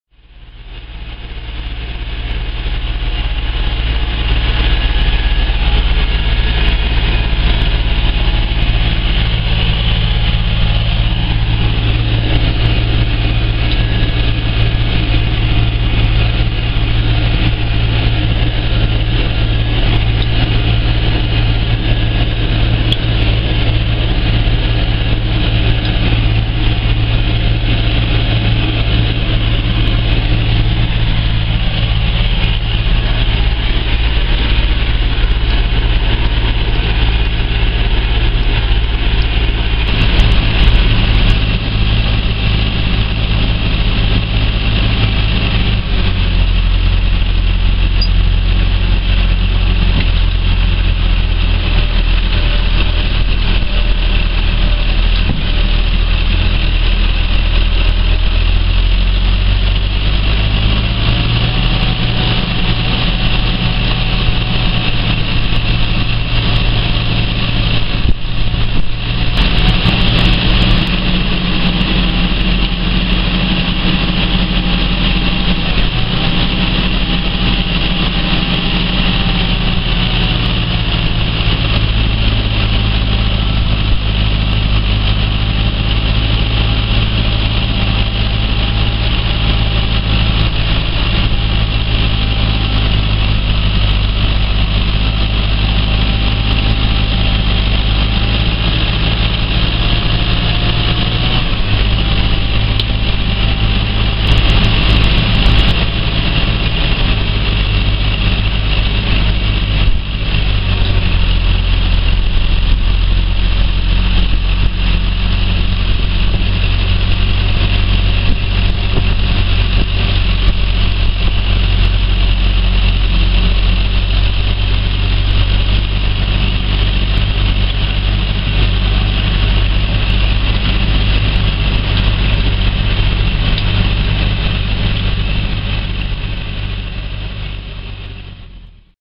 Végre itt a tavasz! Simítózás, 2013.04.11
A szezon első simítózása. Massey Ferguson 4245 + fogasboronás simító